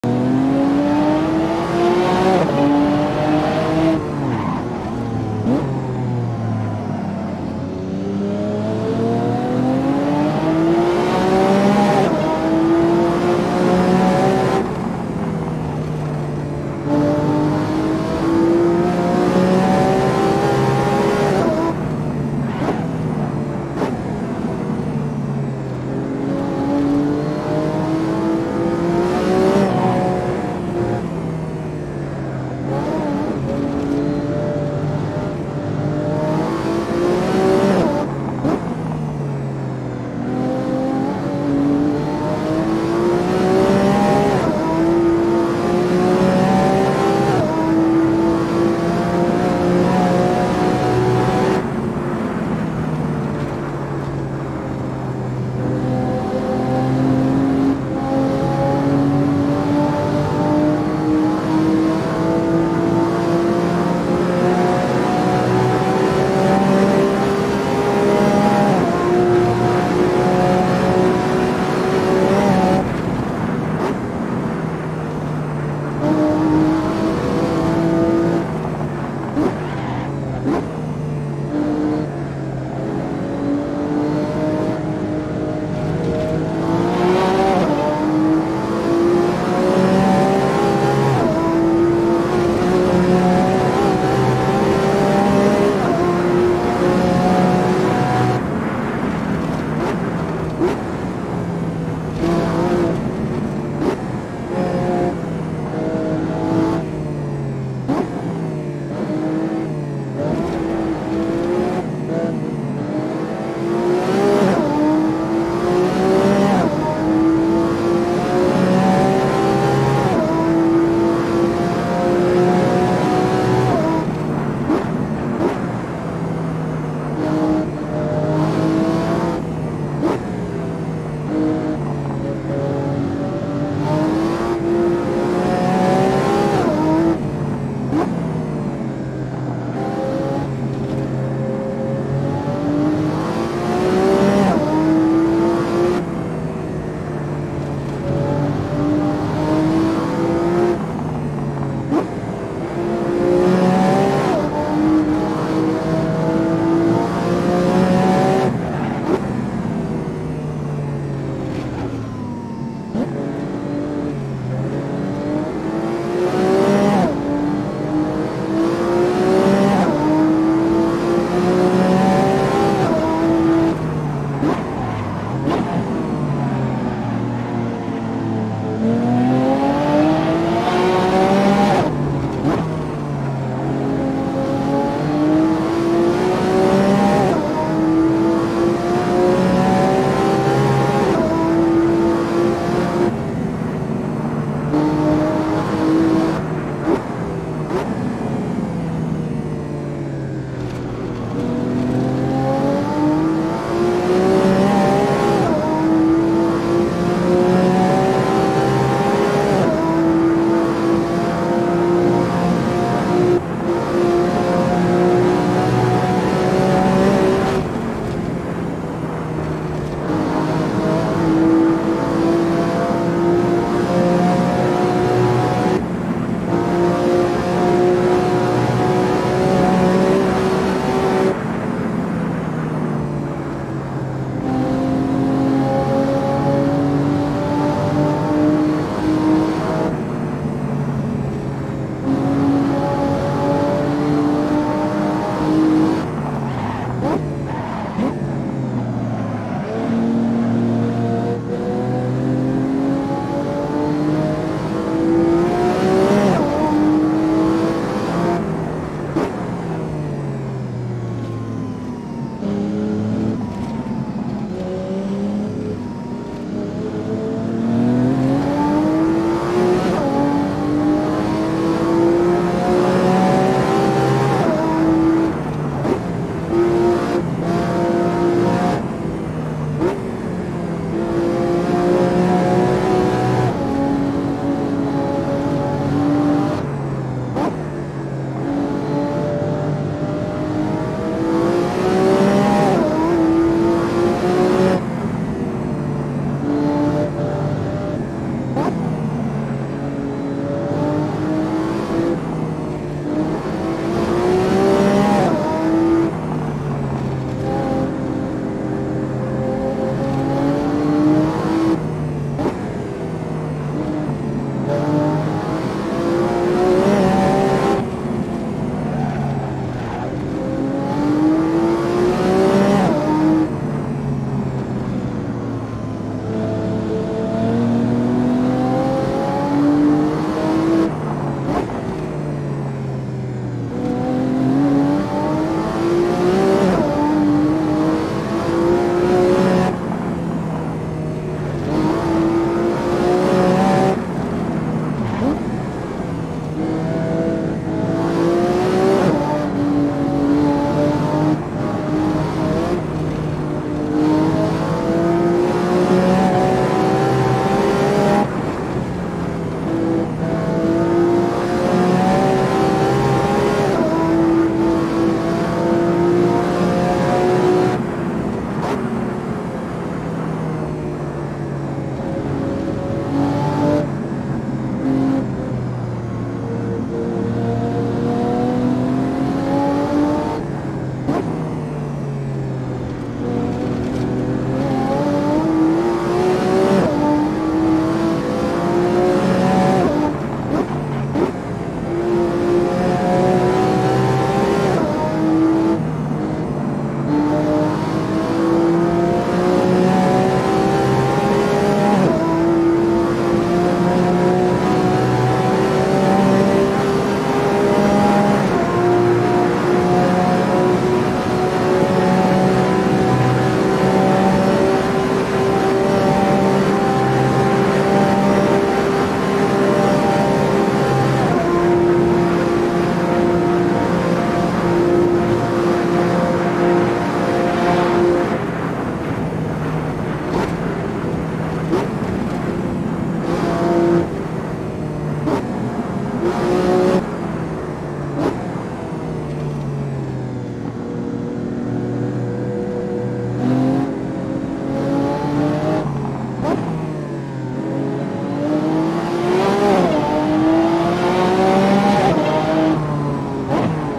Lamborghini LP670sv Engine glory around the nordschliefe
for this round of engine glory I decided to take the screaming v12 of the lamborgini murcielago lp670-4 SV for a spin, this is the result.